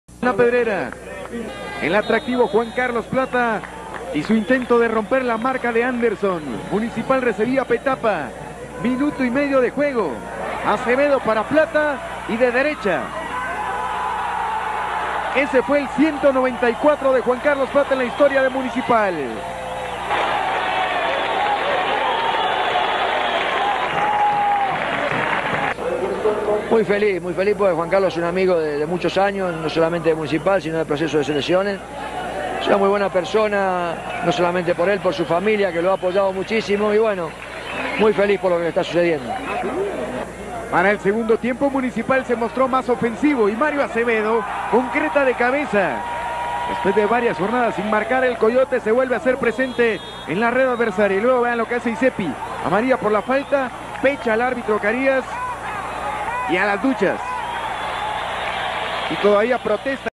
Juan Carlos Plata se convierte en máximo anotador de MUNICIPAL
Gol 195/
Dejando a Julio César Anderson Con 194